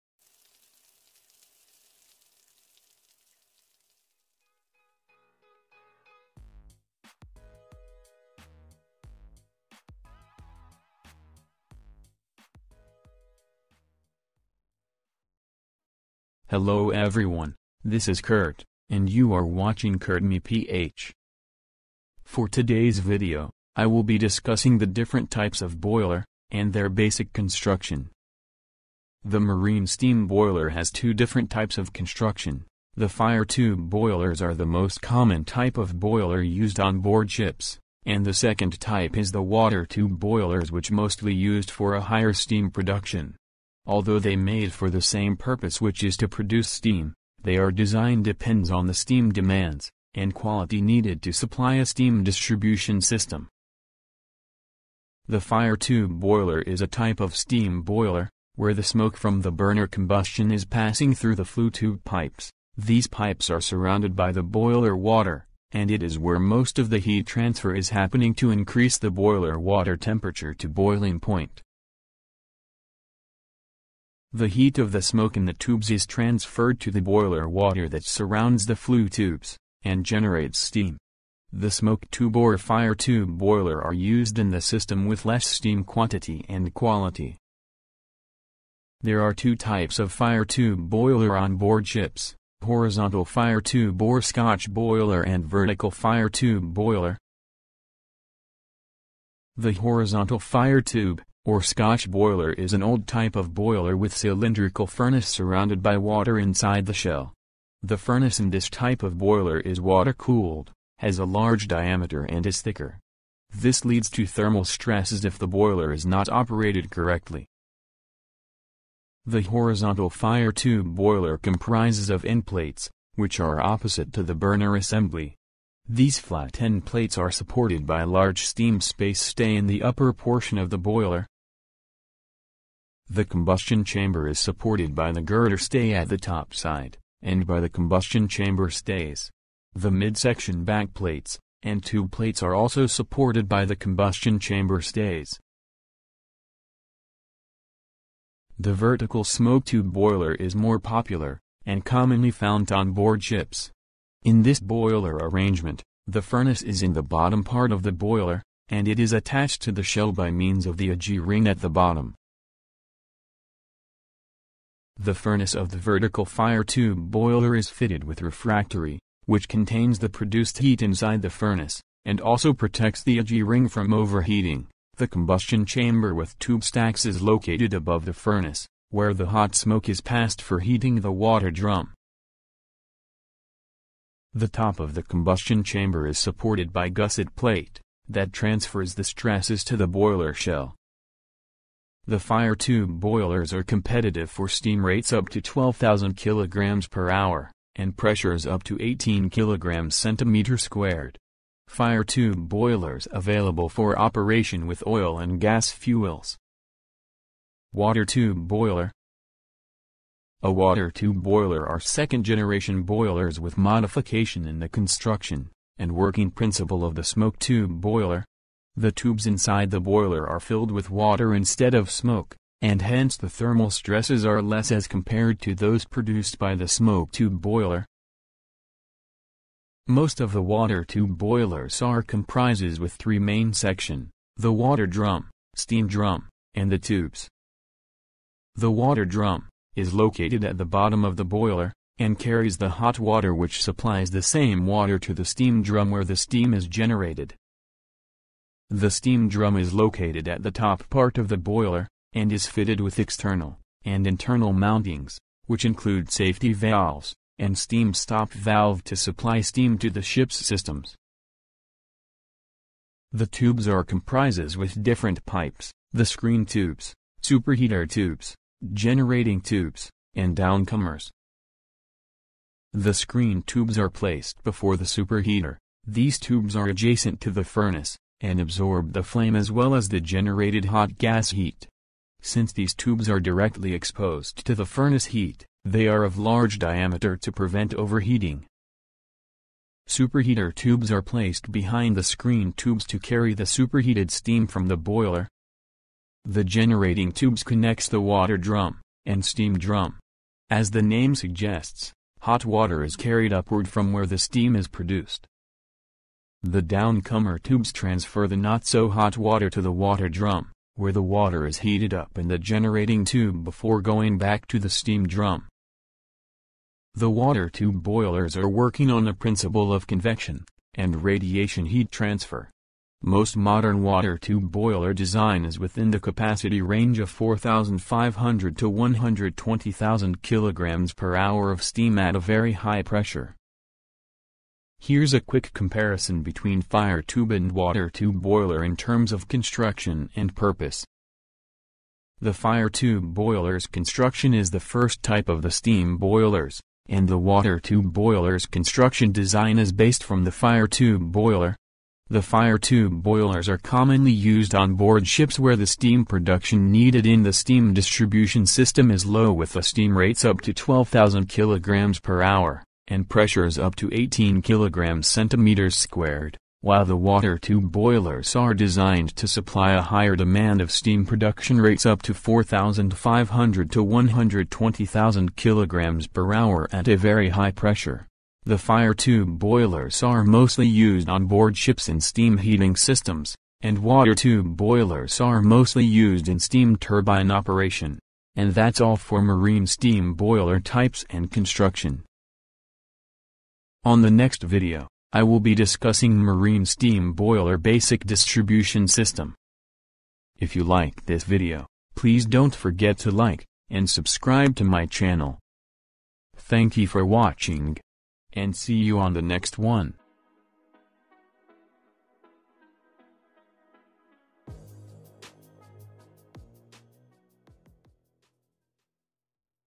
Types of Marine Steam Boiler and Construction | Kurt ME PH
This video is about the Types of Marine Steam Boiler and construction | Kurt ME PH

Marine Steam Boiler Comprises with two different types of construction, the Fire Tube or smoke Tube Boiler and Water Tube Boiler. The Fire Tube Boiler is a type of Marine Boiler where the Smoke from the combustion of the fuel burner is passing inside the flue tube pipes which is surrounded by water and the Water tube boiler is type of Marine Boiler that the water is inside the Tube pipes and is heat from the outside.